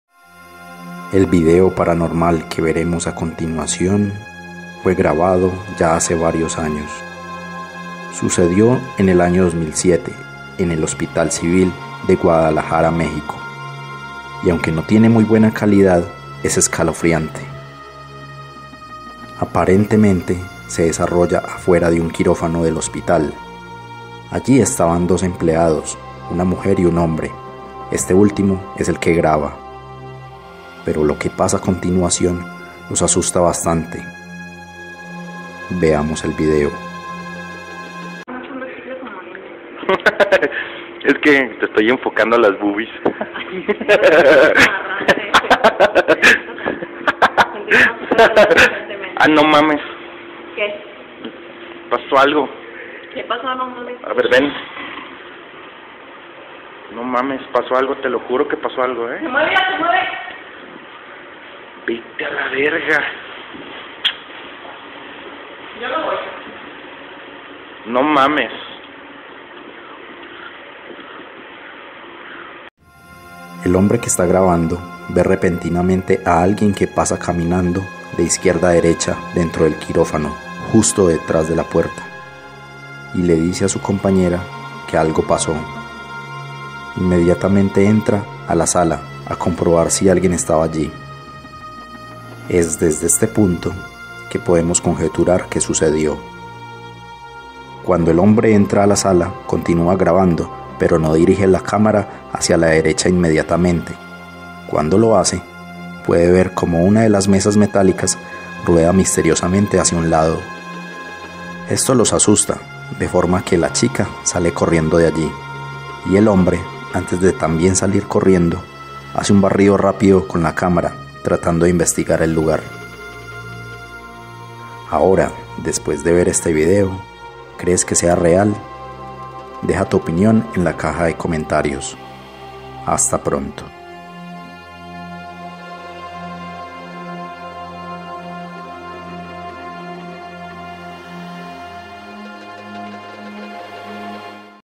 TERRORÍFICA GRABACIÓN DE UN FANTASMA EN UN HOSPITAL
Es la insólita grabación de un fantasma visto en un hospital en México, Ven y descubre si es real o no.